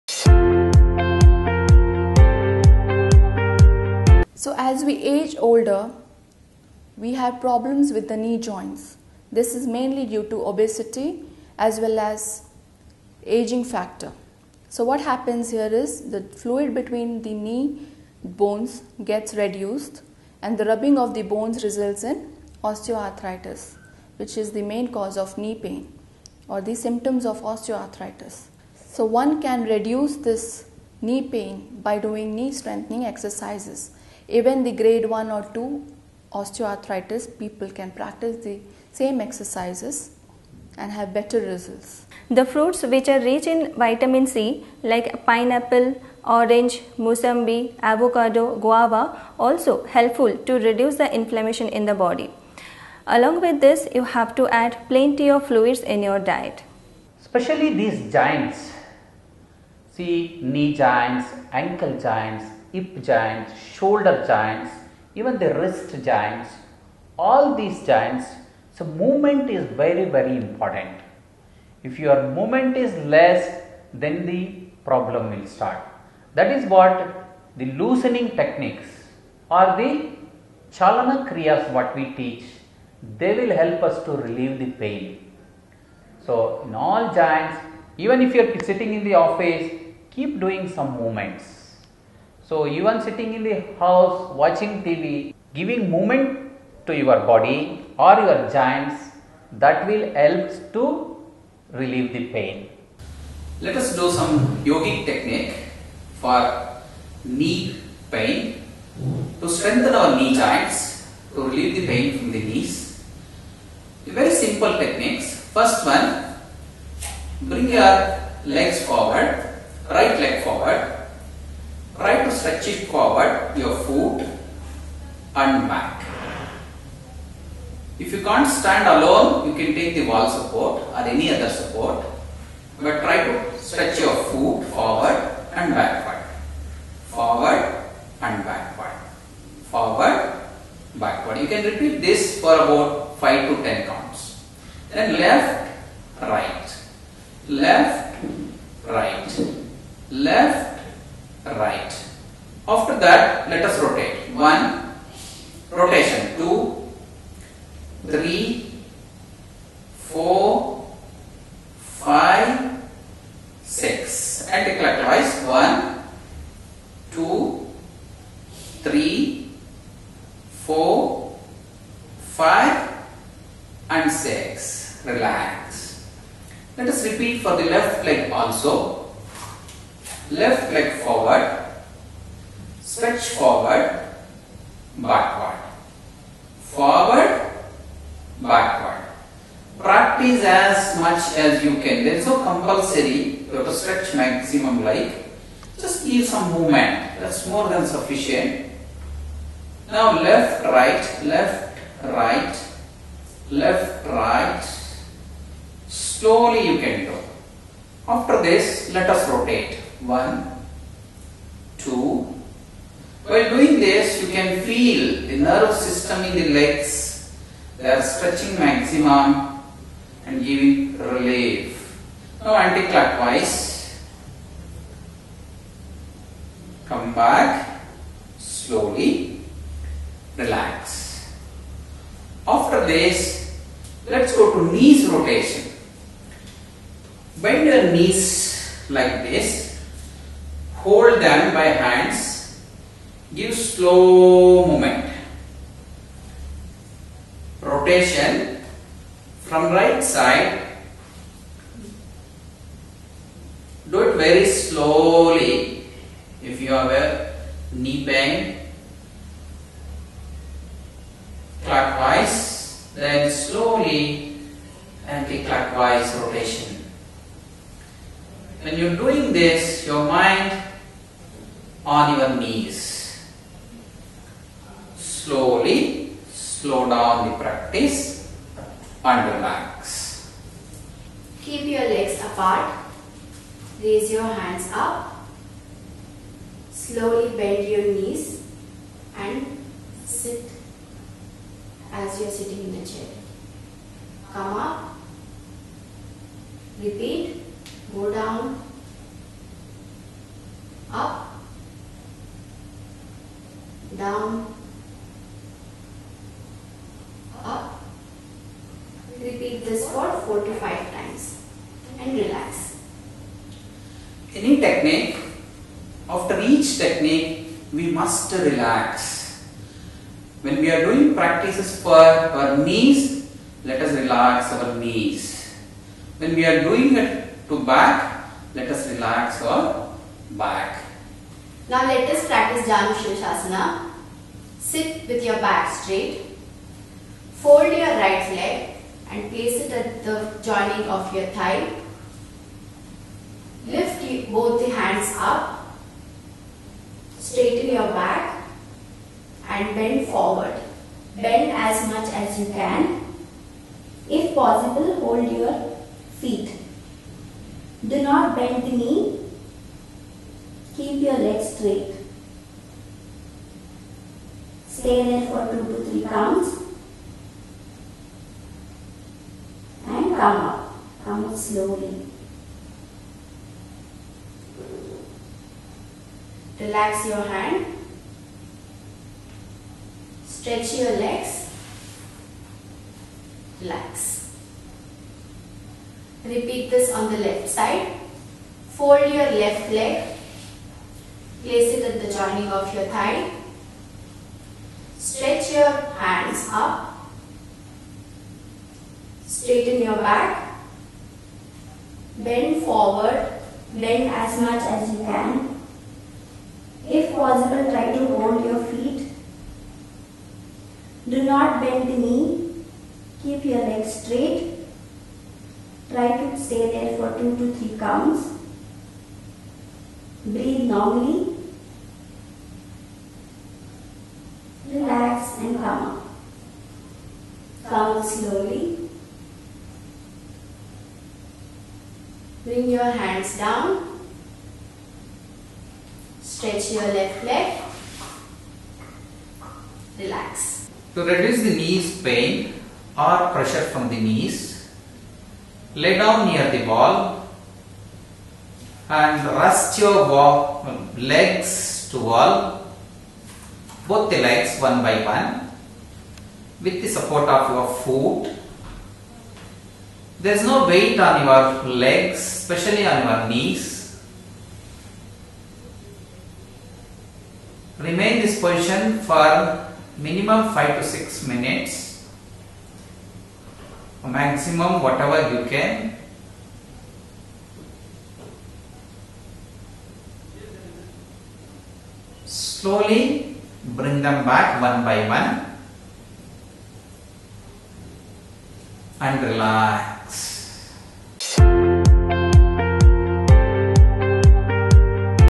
Yoga Exercises for Knee Pain Relief | Manipal Hospitals India
The team of experts at Manipal Hospitals Jayanagar elaborate on the causes of pain in the knee joints, its consequences as well as dietary tips that can help reduce inflammation in the body. This video also shows some simple yoga exercises which can help relieve pain in the knee joints. Watch here to know more!